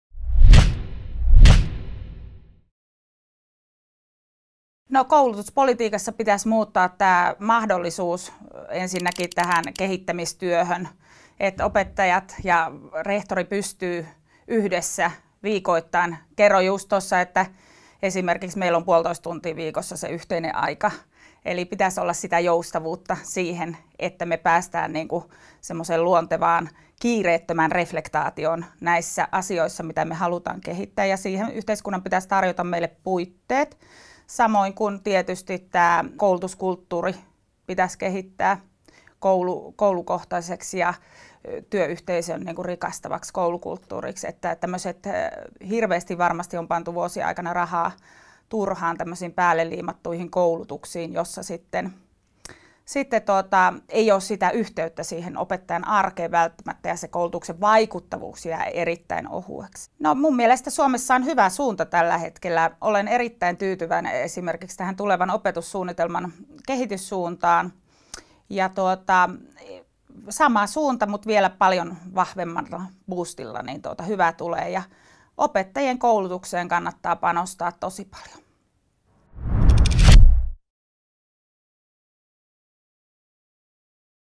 Anna-Mari Jaatinen ja koulutuspolitiikka| HundrED
Rehtori Anna-Mari Jaatinen kertoo, mitä koulutuspolitiikassa pitäisi hänen mielestä muuttaa.